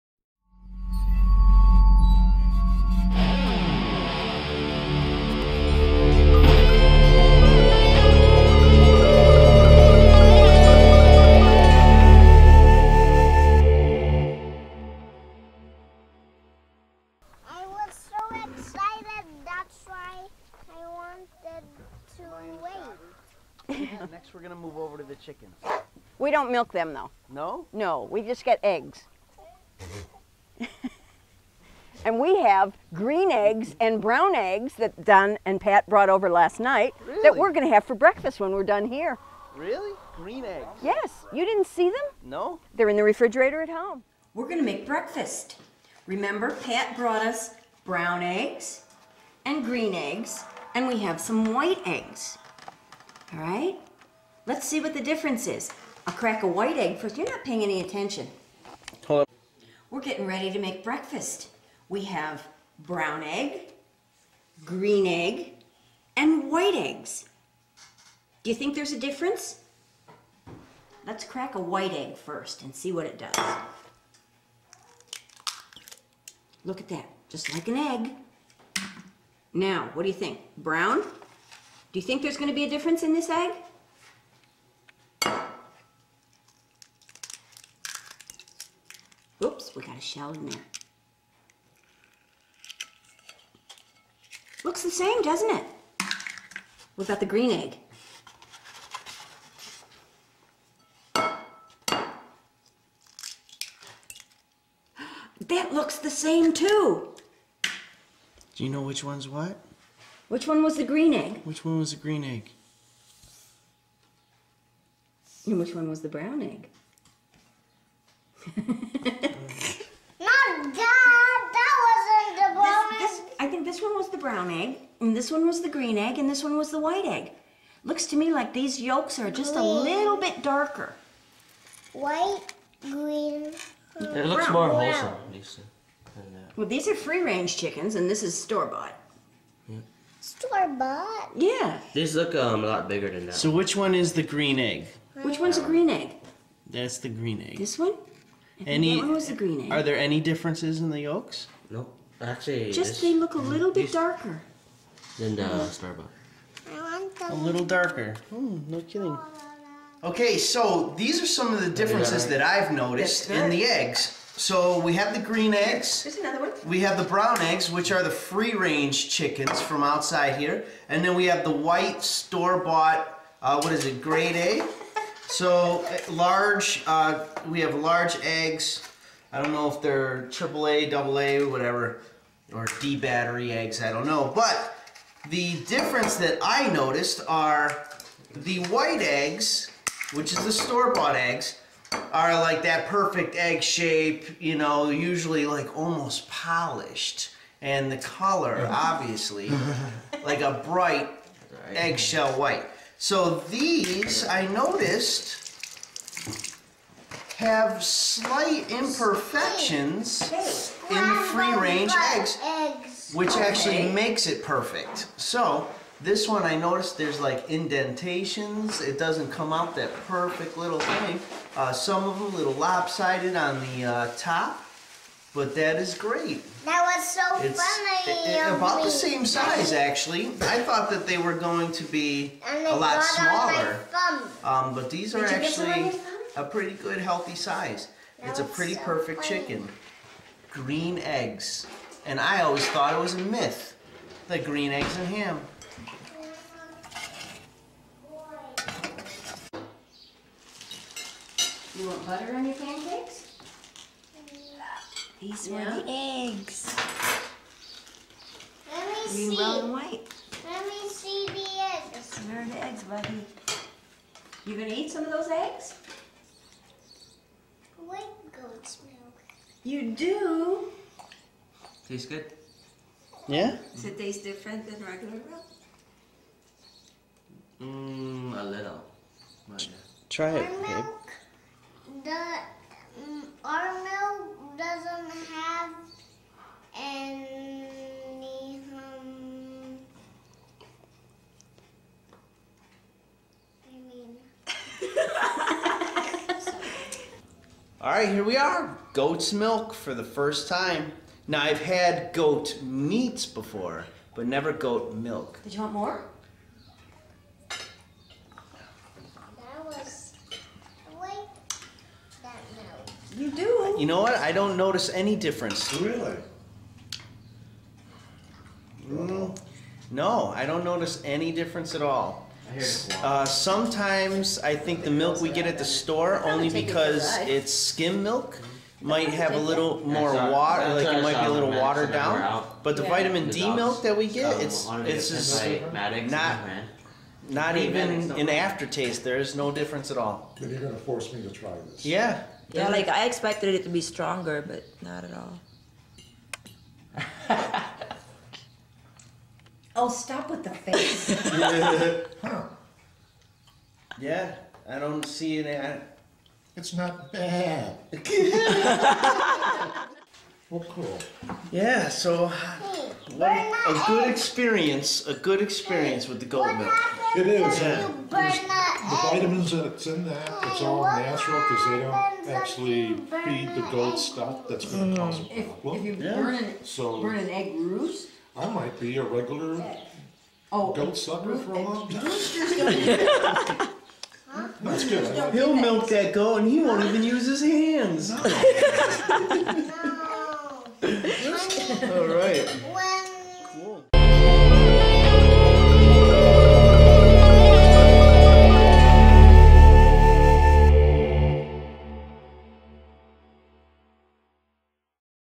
First time for everything
First time for fresh egg's and goat milk.